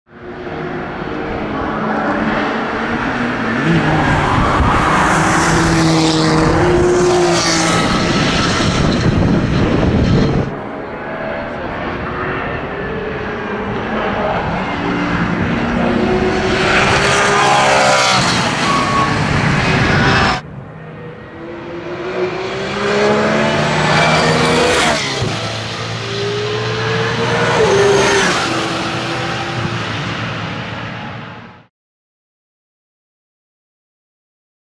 Grello Manthey EMA Porsche 992 GT3-R 2023 Nürburgring Nordschleife NLS1 Test
Here is the new 2023 Porsche 992 GT3-R Grello Manthey EMA at the Friday Practice from NLS Round 1.